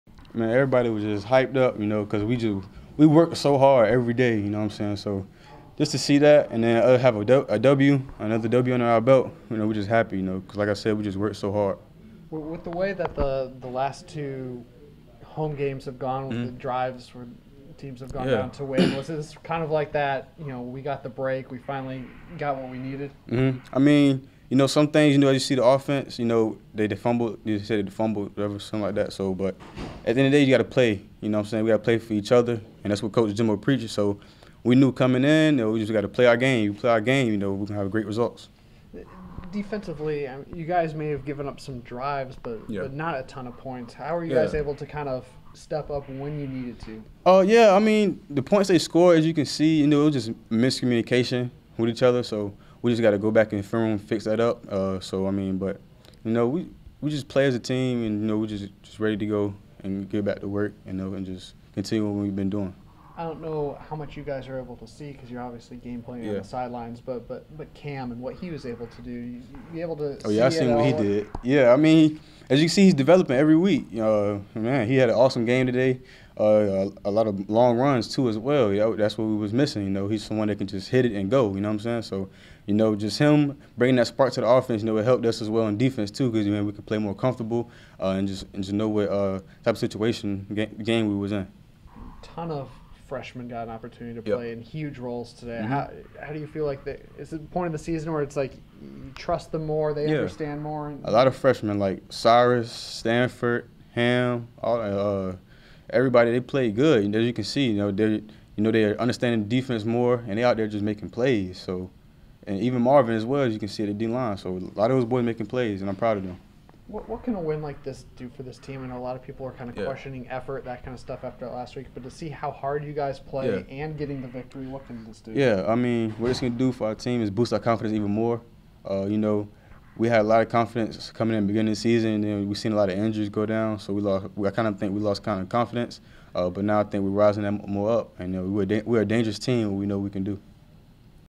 Levonta Taylor Postgame: Syracuse
Levonta Taylor speaks with the media following the win against Syracuse.